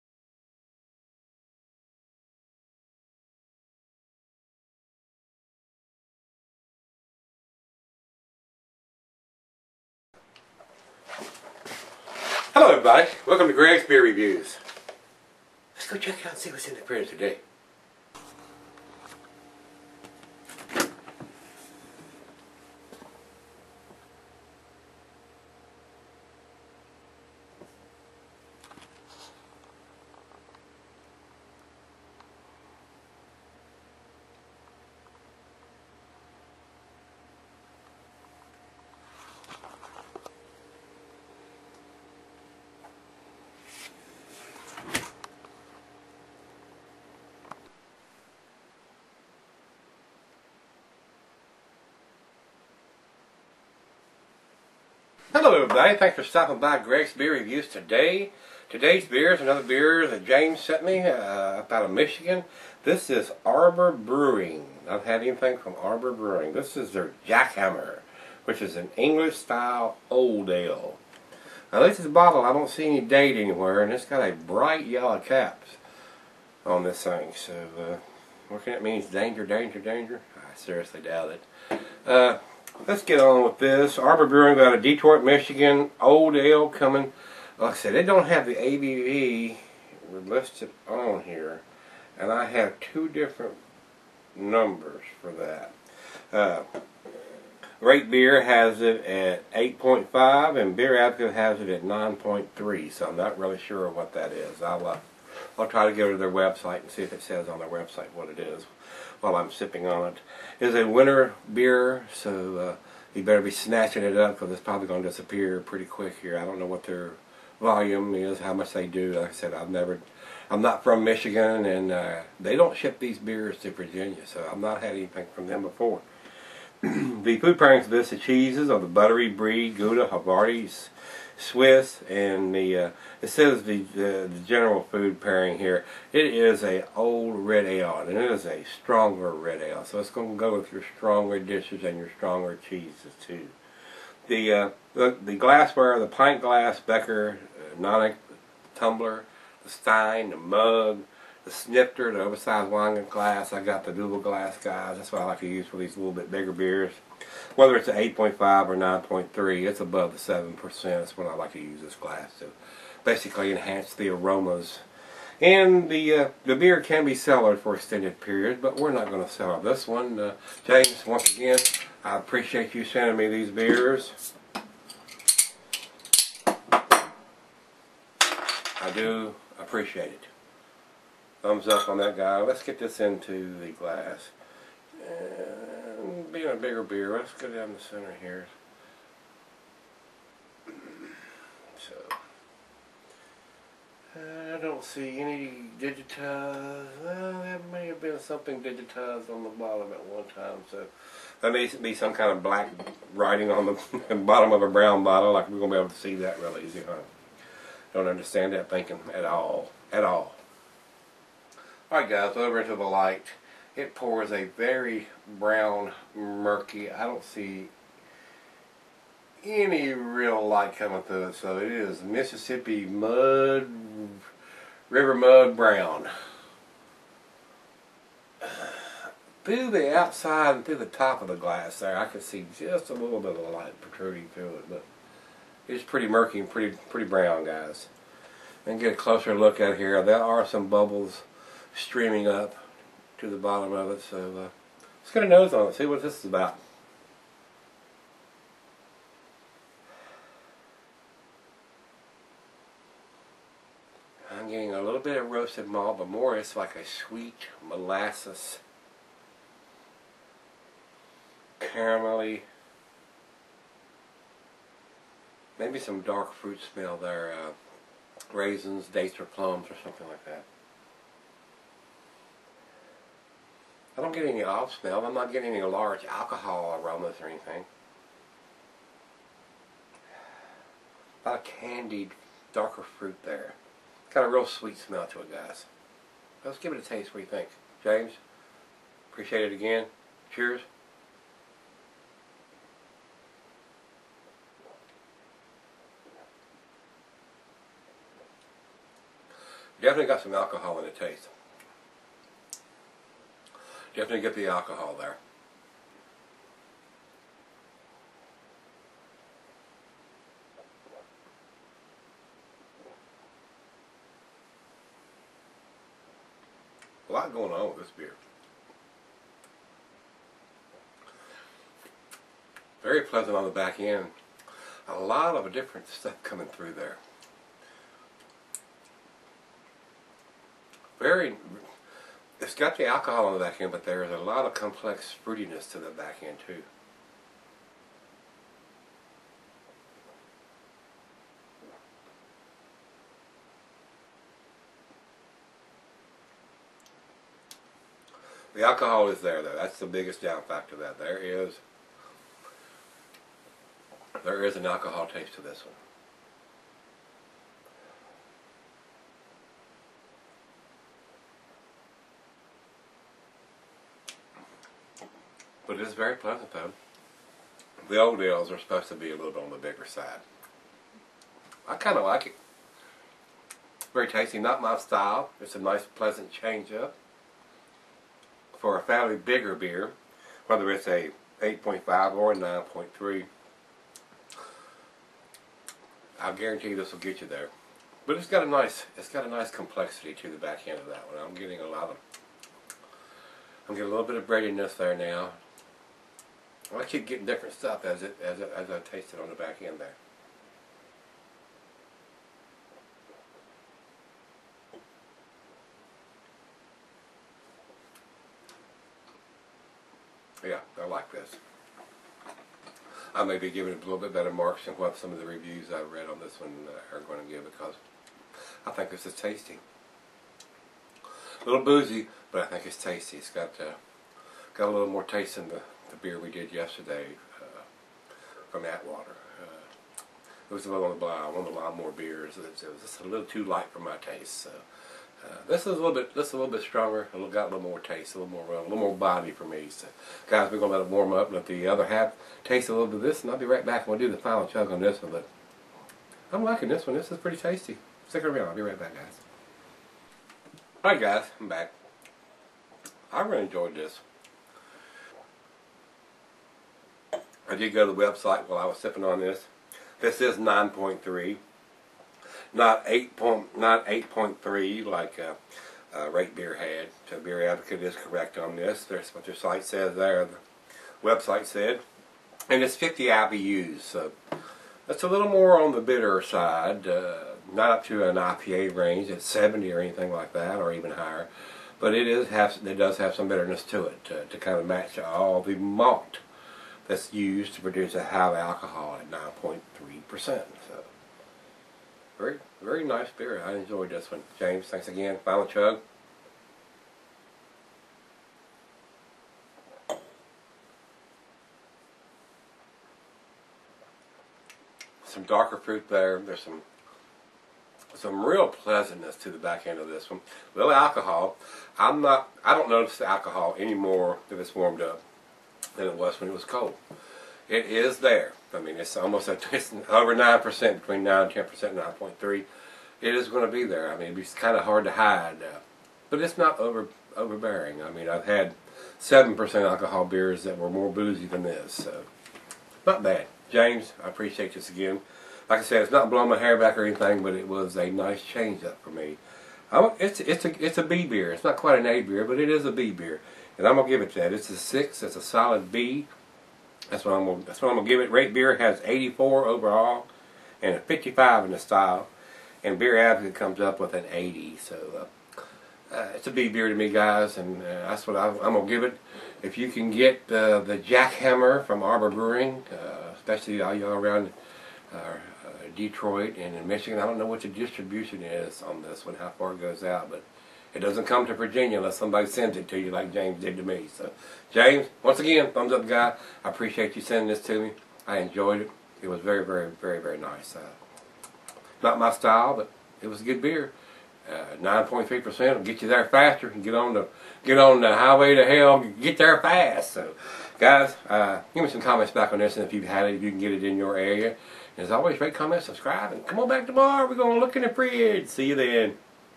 Beer Review # 618 Arbor Brewing Jackhammer Old Ale
Arbor Brewing Jackhammer Old Ale  is the beer reviewed -
BEER SCALE
1- UNDRINKABLE
2- F
3- D
4- C
5- B-
6- B
7- B+
8- A-
9- A
10- A+
CHEERS!